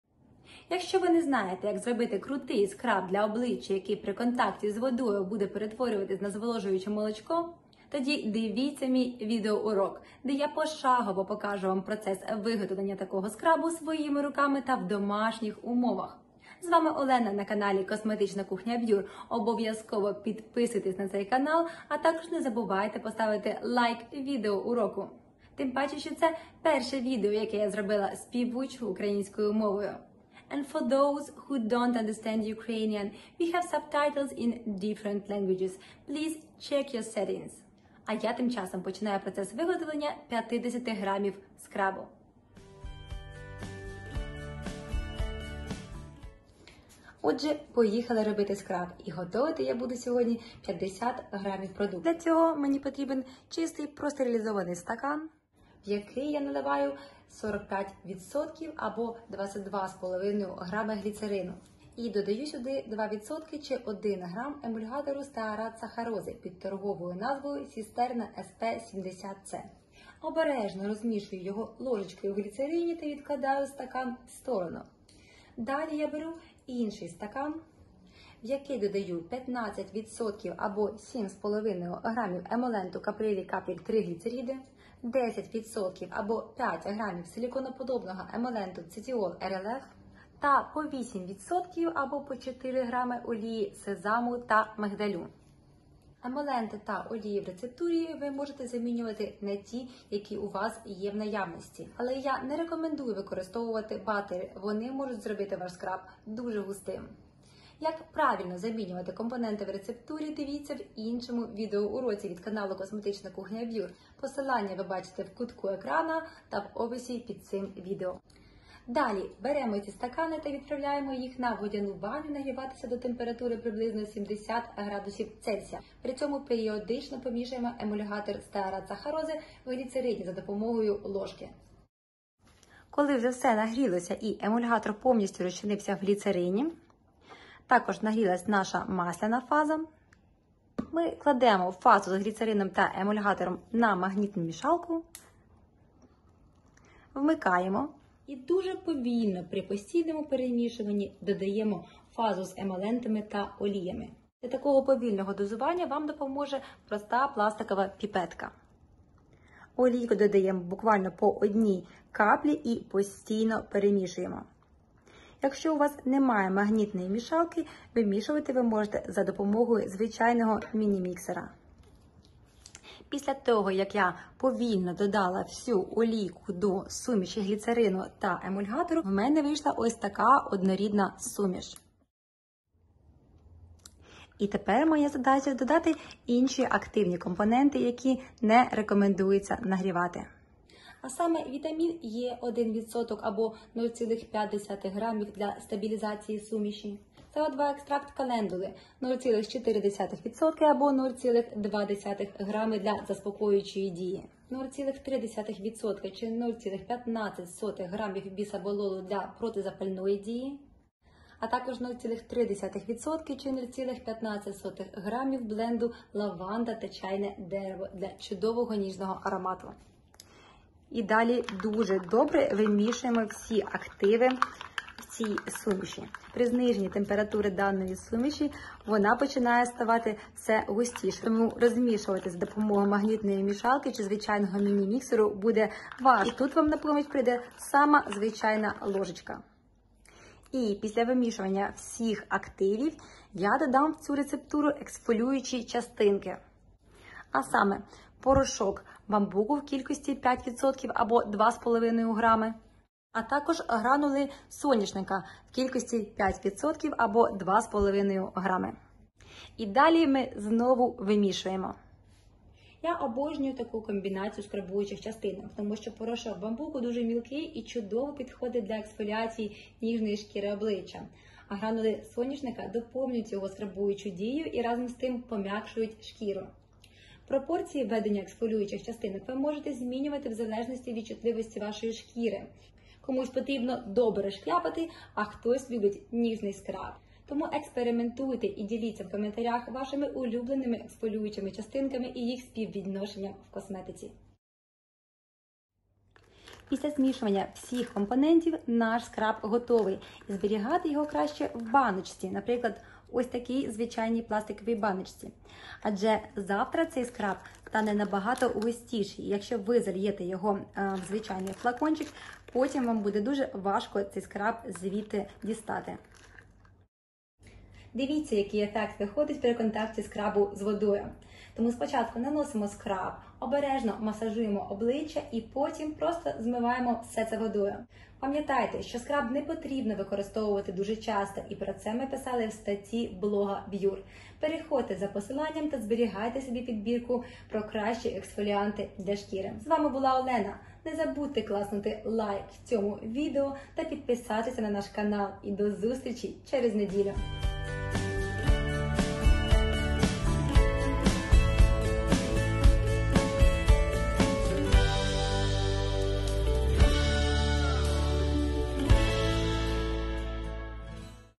ЯК ЗРОБИТИ ОЛЕОГЕЛЬ СКРАБ ДЛЯ ОБЛИЧЧЯ: рецепт своїми руками
Не знаєте, як зробити крутий скраб для обличчя на основі емолентів та олії, який при контакті зі шкірою буде перетворюватись на зволожуюче молочко? Тоді дивіться цей відео урок, де ми пошагово покажемо вам процес виготовлення такого скрабу своїми руками в домашніх умовах.

РЕЦЕПТ НА 50 грамів:

Фаза 1 - 47%
Гліцерин - 45% або 25 г
Емульгатор Sisterna SP-70C - 2% або 1 г

Фаза 2 - 41%
Емолент Caprylic/capric triglycerides - 15% або 7,5 г
Емолент Cetiol RLF - 10% або 5 г
Олія мигдальна раф. - 8% або 4 г
Олія кунжутна раф. - 8% або 4 г

Фаза 3 - 12%
Вітамин Є - 1% або 0,5 г
СО2 екстракт календули - 0,4% або 0,2 г
Бісаболол - 0,3% або 0,15 г
Бленд Лаванда та чайне дерево - 0,3% або 0,15 г
Порошок бамбука - 5% або 2,5 г
Гранули соняшника - 5% або 2,5 г

Як перевести відсотки в грами?
Приклад: загальна вага готового засобу 15 г, відсоток введення компонента 3%, отже, необхідно 3% помножити на 15 г і розділити на 100 і отримаємо 0,45 грамів компонента.